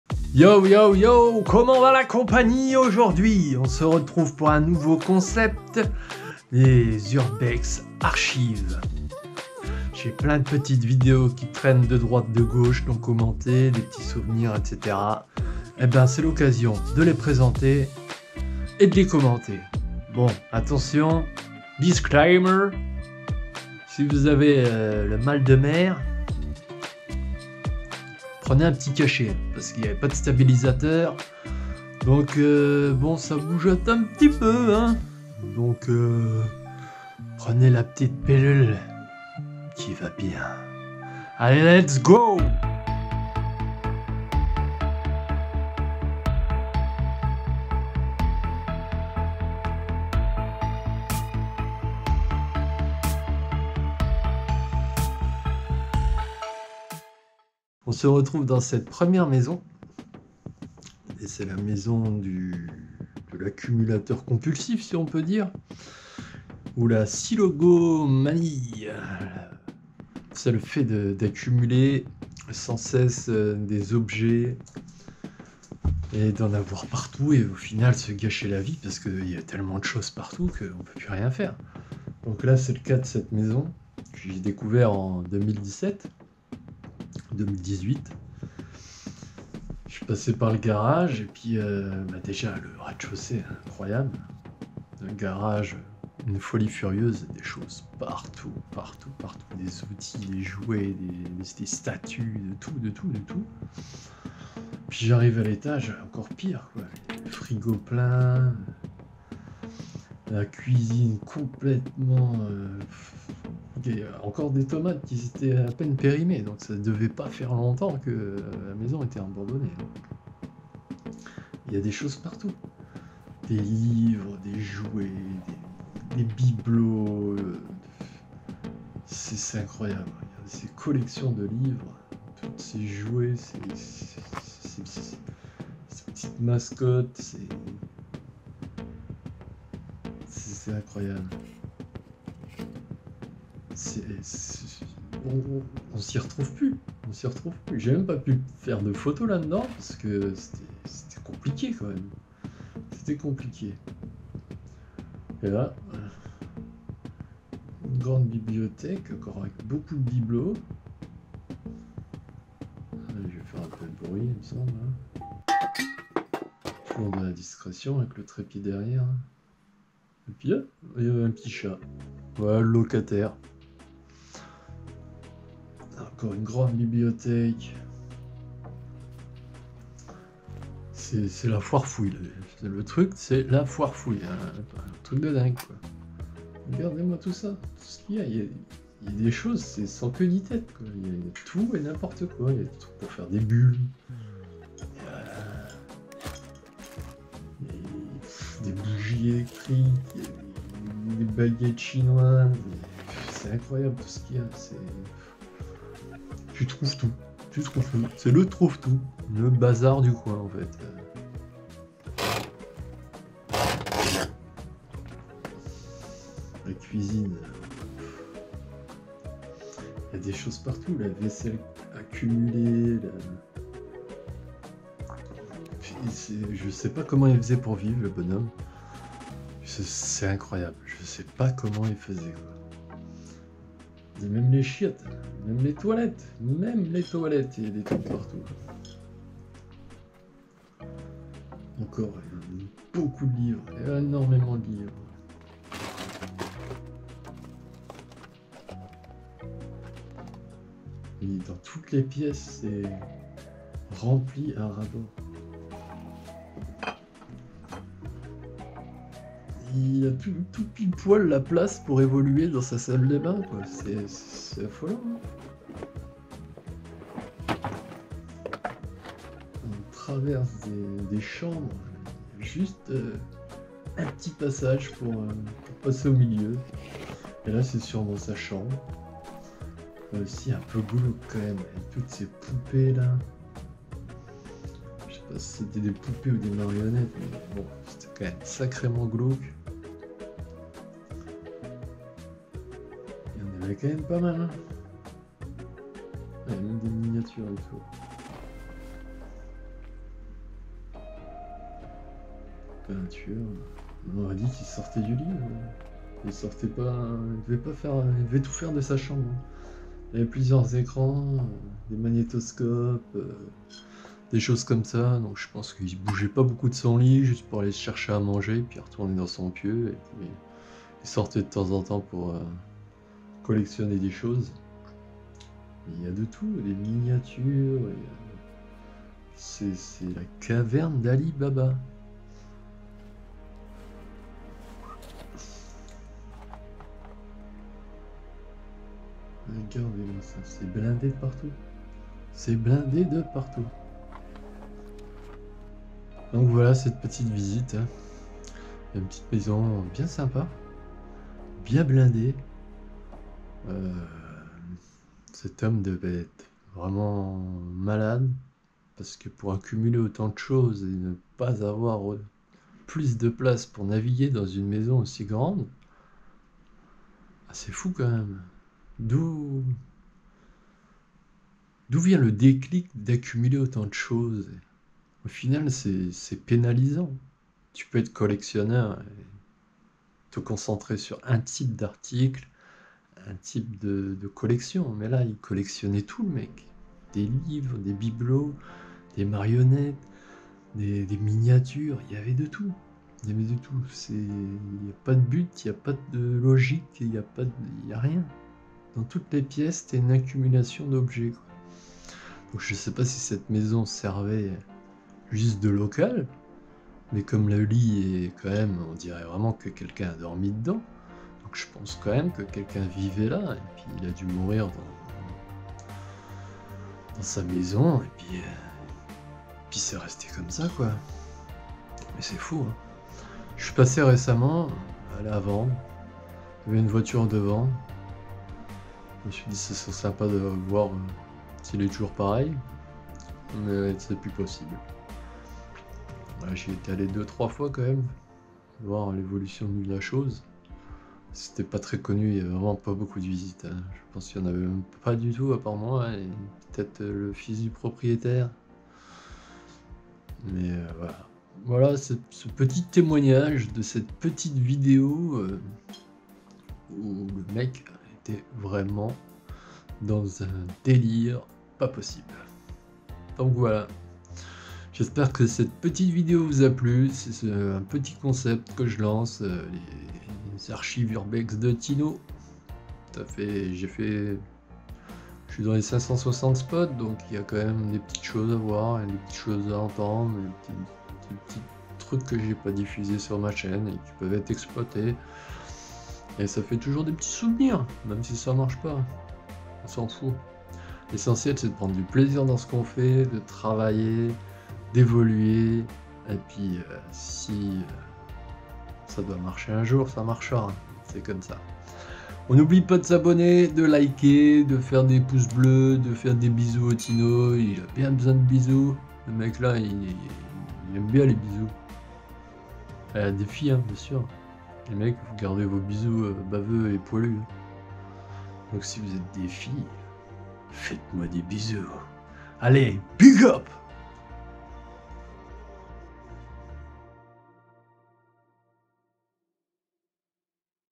Une accumulation incroyable dans la Maison Diogène - Urbex archives 03
Une personne atteinte du syndrome de Diogène, qui a accumulé des milliers d'objets dans sa maison. A un tel point qu'il ne reste pas beaucoup de place pour ce déplacer.